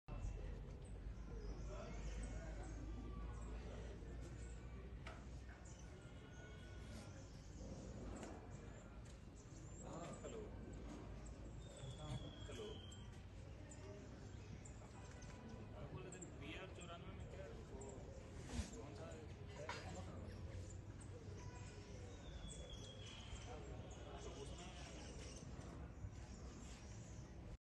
Mount kanchanjanga, India's highest peak of Himalayas. Mesmerizing view from Tiger hill, Darjeeling.
video credit- MD. FAISAL ALI DAO